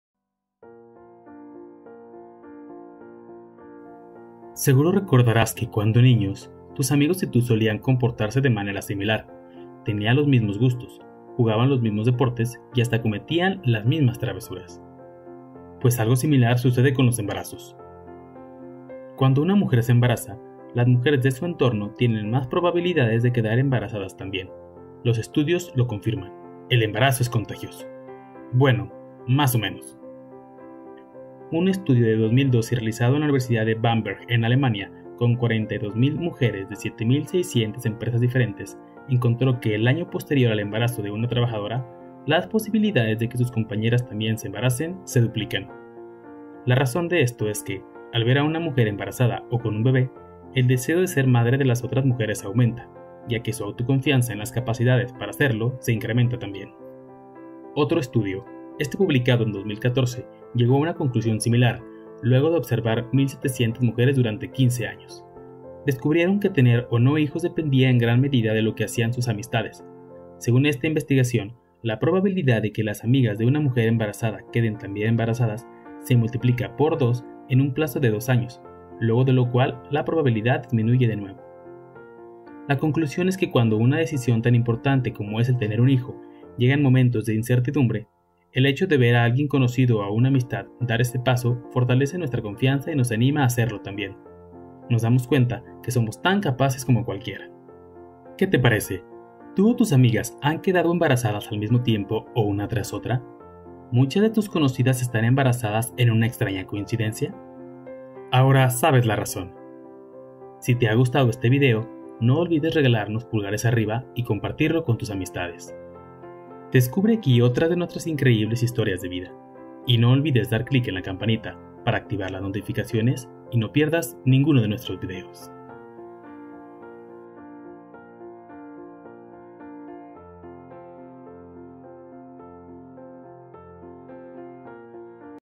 El embarazo es "contagioso" los estudios lo confirman!!
¿Te has dado cuenta que cuando una mujer se embaraza sus amigas se embarazan tambie? Esta es la increíble y sorprendente razón!❤️